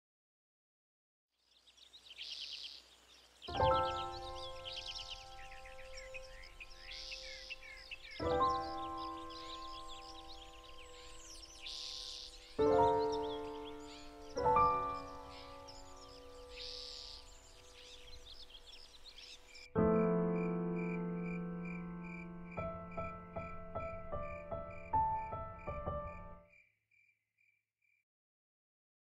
An Ordinary Day
A small animation I did for my first assignment in Motion Graphics at Santa Barbara City College